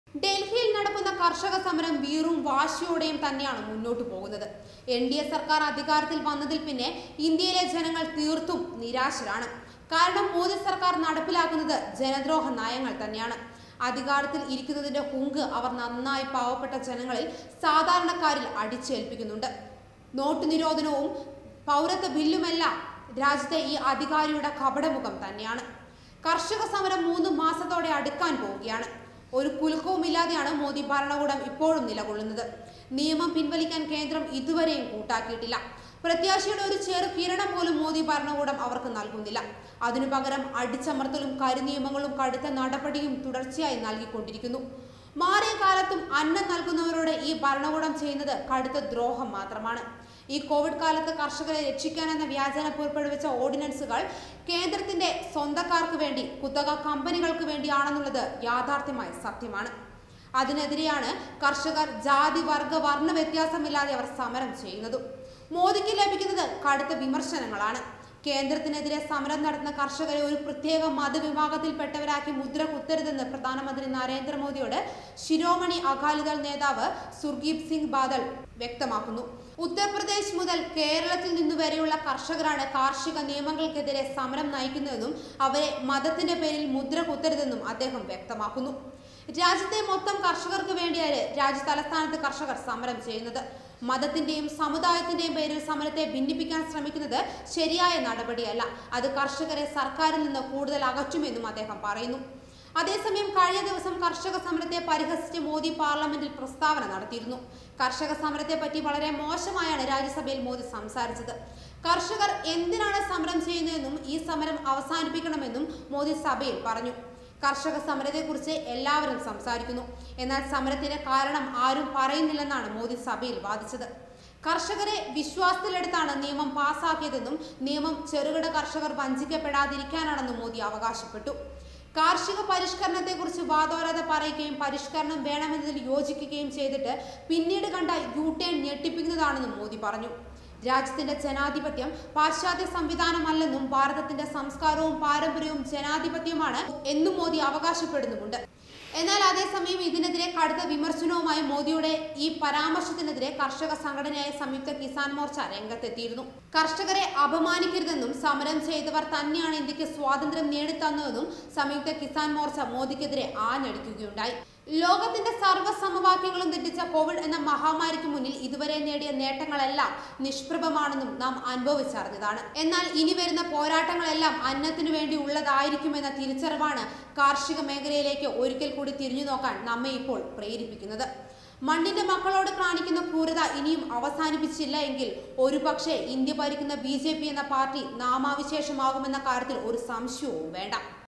കർഷക സമരം തകർക്കാൻ അവസാന അടവ്. ബിജെപി യുടെ നെറികേടിനെതിരെ താക്കീതുമായി പഴയ സഖ്യകക്ഷി
ഡൽഹിയിൽ നടക്കുന്ന കർഷകസമരം വീറും വാശിയോടെയും തന്നെയാണ് മുന്നോട്ട് പോകുന്നത്.എൻ ഡി എ സർക്കാർ അധികാരത്തിൽ വന്നതിൽ പിന്നെ ഇന്ത്യയിലെ ജനങ്ങൾ തീർത്തും നിരാശരാണ്.കാരണം മോഡി സർക്കാർ നടപ്പിലാക്കുന്നത് ജനദ്രോഹ നയങ്ങൾ തന്നെയാണ്.അധികാരത്തിൽ ഇറിക്കുന്നതിന്റെ ഹുങ്ക് അവർ നന്നായി പാവപ്പെട്ട ജനങ്ങളിൽ ,സാധാരണകാറിൽ അടിച്ചേൽപ്പിക്കുന്നുണ്ട്.നോട്ടു നിരോധനവും ,പൗരത്വ ബില്ലുമെല്ലാം രാജ്യത്തെ ഈ അധികാരികാരിയുടെ കപടമുഖം തന്നെയാണ്.കർഷക സമരം മൂന്ന് മാസത്തോടെ അടുക്കാൻ പോകുകയാണ്.ഒരു കുലുക്കവും ഇല്ലാതെയാണ് മോഡി ഭരണകൂടം ഇപ്പോഴും നിലകൊള്ളുന്നത്. നിയമം പിൻവലിക്കാൻ കേന്ദ്രം ഇതുവരെയും കൂട്ടാക്കിയിട്ടില്ല.പ്രത്യാശയുടെ ഒരു ചെറുകിരണം പോലും മോഡി ഭരണകൂടം അവർക്ക് നൽകുന്നില്ല.അതിനു പകരം അടിച്ചമർത്തലും കരിനിയമങ്ങളും കടുത്ത നടപടികളും തുടർച്ചയായി നൽകിക്കൊണ്ടിരിക്കുന്നു.മാറിയ കാലത്തും അന്നം നല്കുന്നവരോട് ഈ ഭരണകൂടം ചെയ്യുന്നത് കടുത്ത ദ്രോഹം മാത്രമാണ്